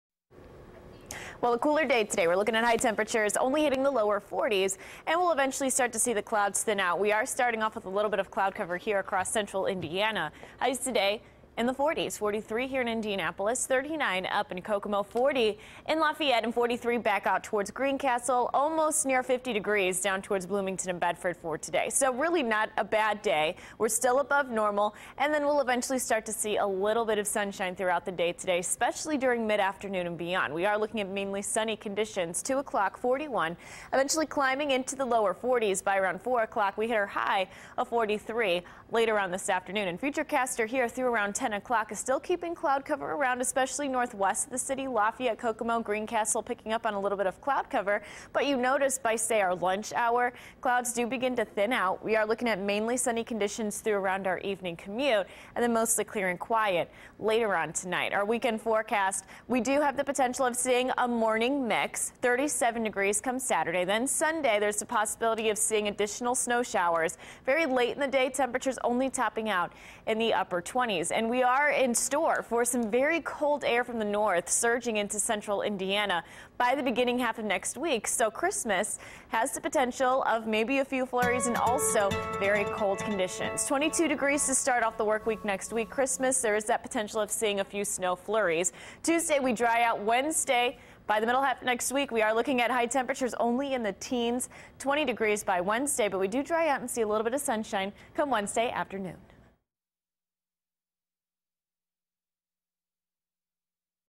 Wednesday's Forecast
Cooler but still above normal with highs in the 40s and mostly sunny skies.